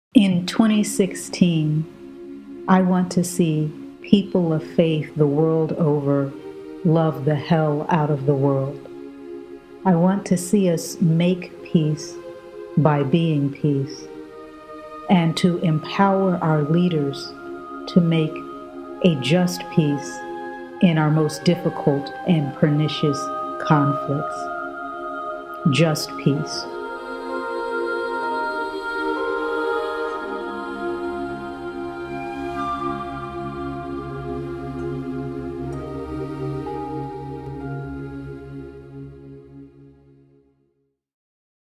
Just Peace
1 // What would it look like to “love the hell out of the world”?
2 // In what “difficult and pernicious conflicts” do you hope to see peace?

Spoken & Written by Will Gafney